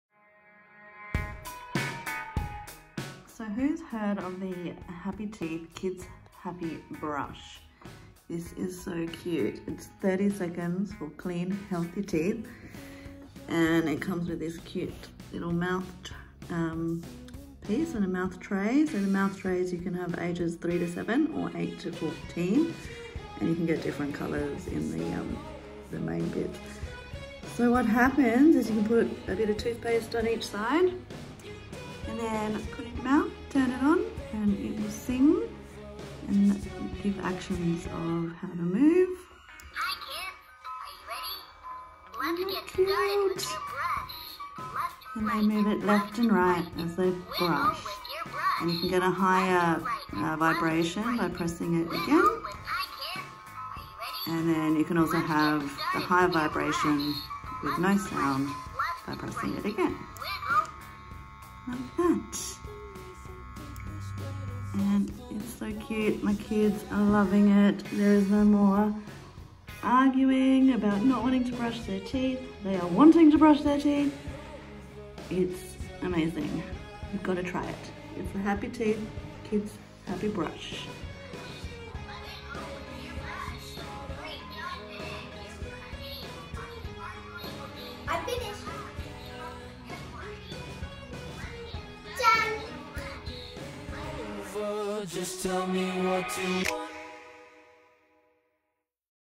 Kids Happi Teeth Happi Brush
Have you seen this cute kids Happi Brush from Happi Teeth? Getting your kids to clean their teeth has never been so easy!! 🥰